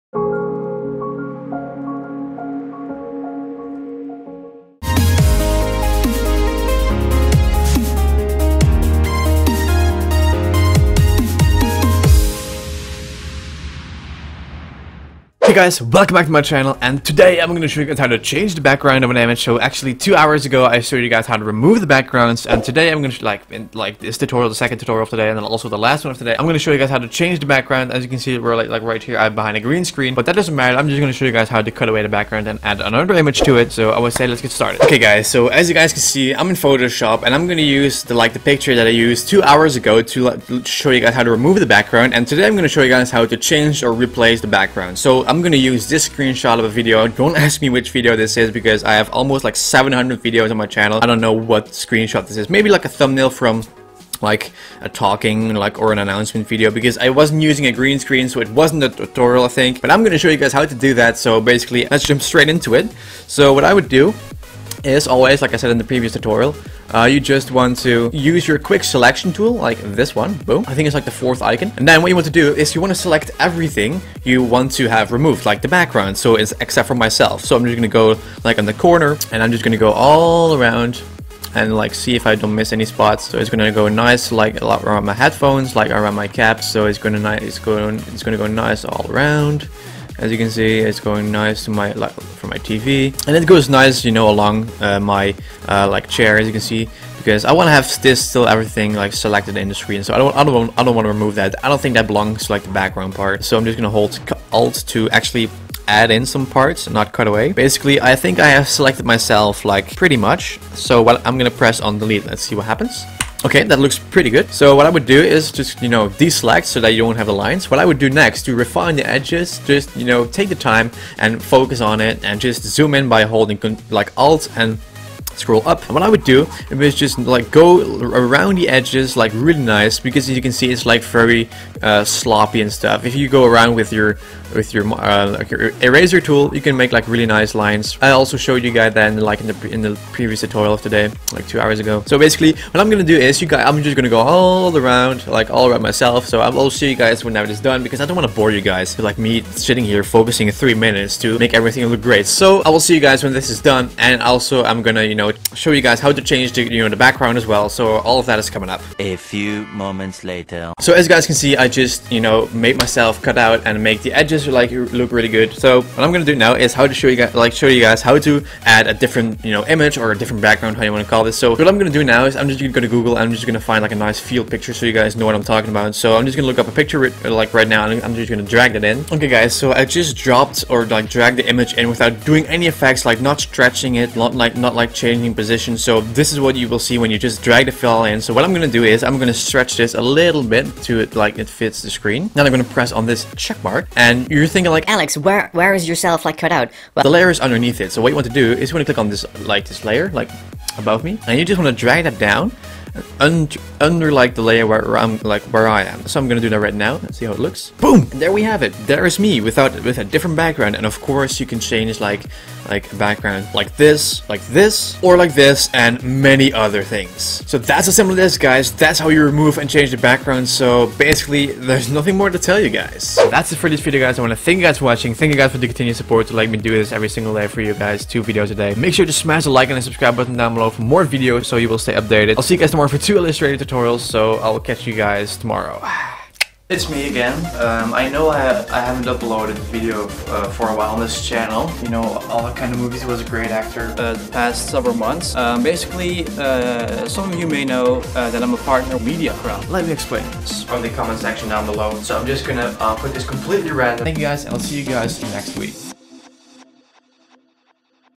How To Change The Background Of An Image EASILY! - Adobe Photoshop CC - Tutorial #6
- - In this video I'll be showing you how to change the background of an image. It's done in the photo editing software called Photoshop CC 2014. You can use Adobe Photoshop CC, CS4, CS5, and CS6.

~ JustAlexHalford